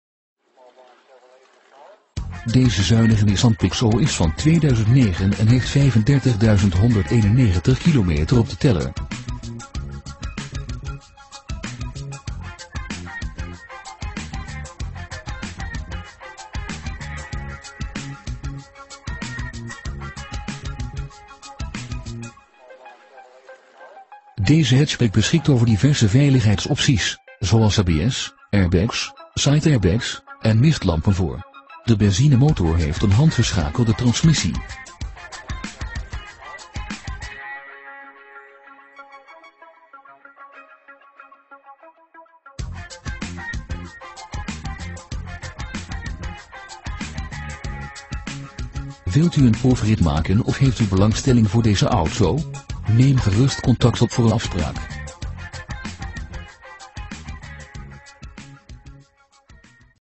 Nissan Pixo 1.0 Acenta
88-JTR-4 35.191 km
  Prijs:     € 5.950
  Merk:     Nissan
  Model:     Pixo
  Type:     1.0 Acenta
  Carrosserievorm:     Hatchback (4 drs)
  Bouwjaar:     2009
  Transmissie:     handgeschakeld
  Kleur:     Licht wit
  BTW/Marge:     marge
  Brandstof:     benzine
* Achterruitwisser(s)
* Airbags
* Mistlampen voor
* Side-airbags voor
* Stuurbekrachtiging

Deze auto bieden wij u aan tegen een scherpe meeneemprijs, exclusief garantie. Wel verzorgen we een nieuwe APK bij aflevering.

Aanbieder
Van Dorst Breda B.V.
Crogtdijk 54
4825BA BREDA